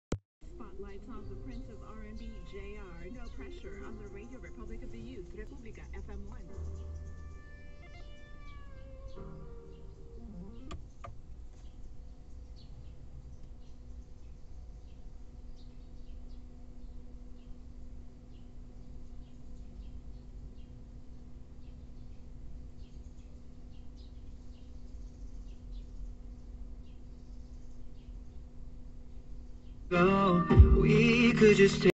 How to reset infotainment system (s presso)
If you are experiencing troubles with the system like unable to connect bluetooth or unit is not turning off, this would do the trick. 👍